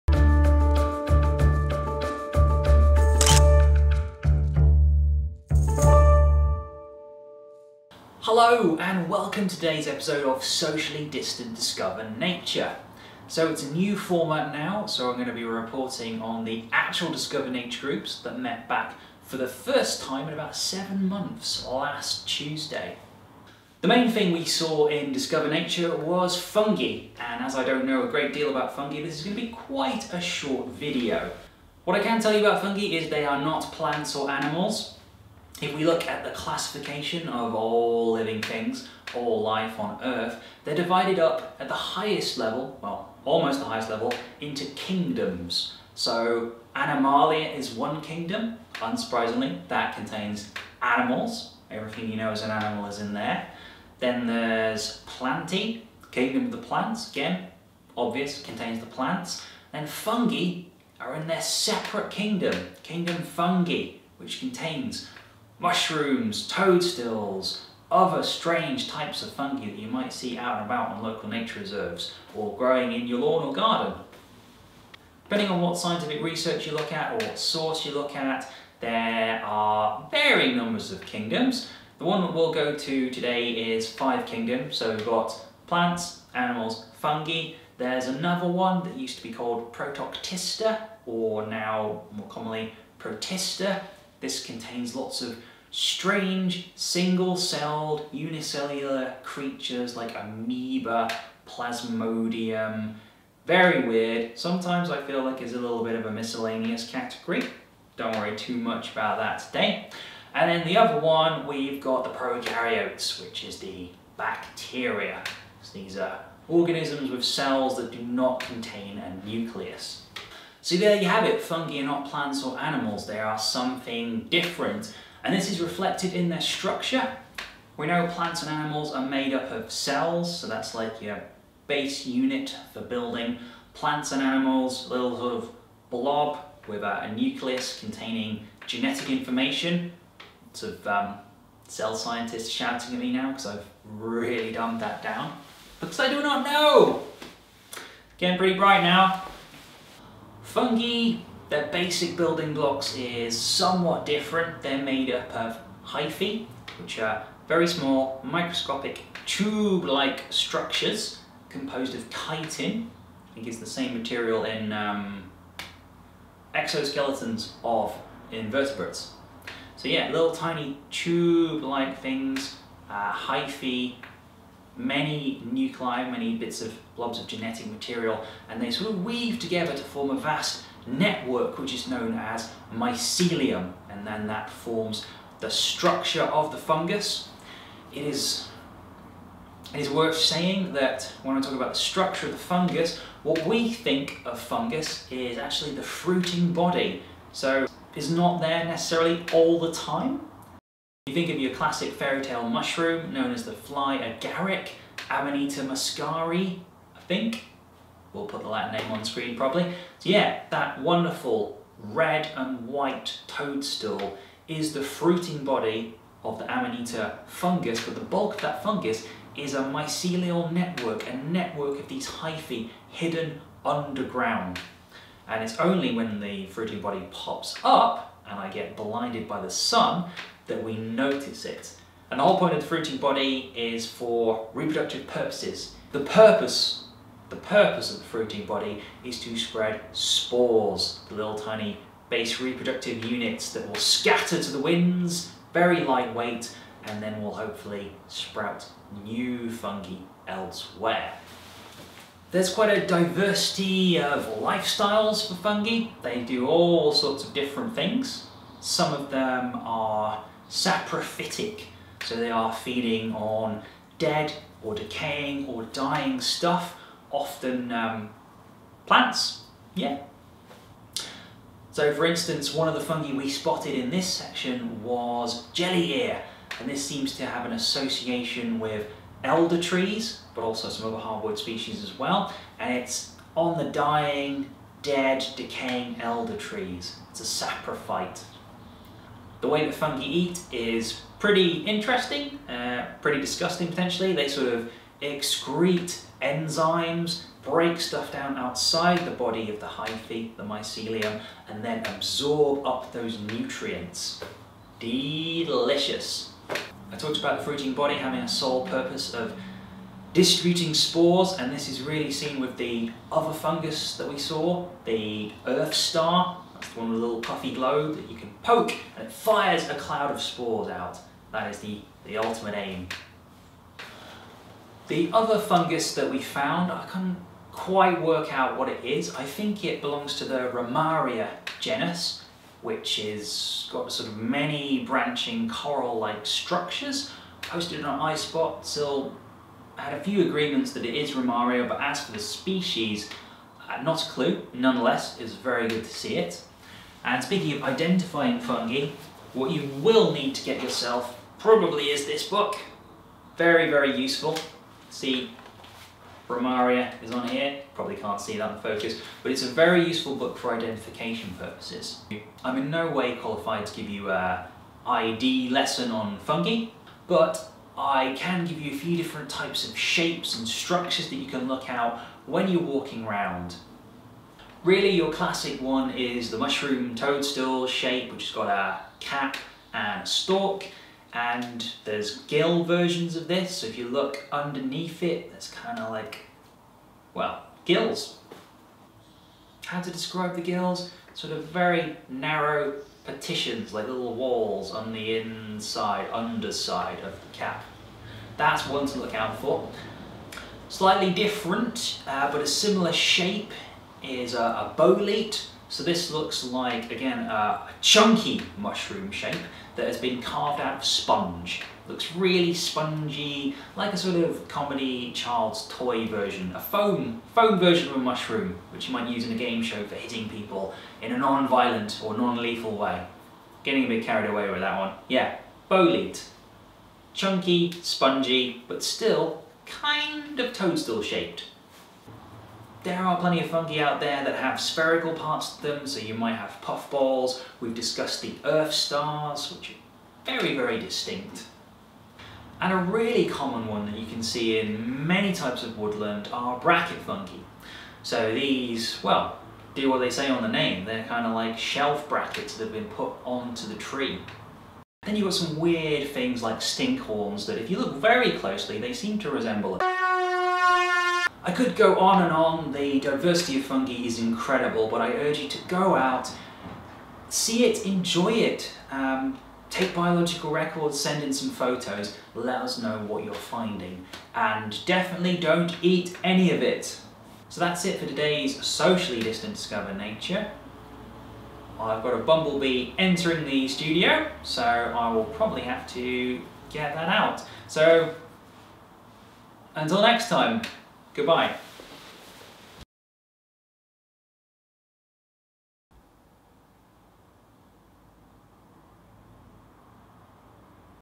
Fantastic Fungi! - Discover Nature #28
In this week’s episode we talk about the varied types of fungi that can be found at St Nicks and beyond. Also a background dog is present throughout…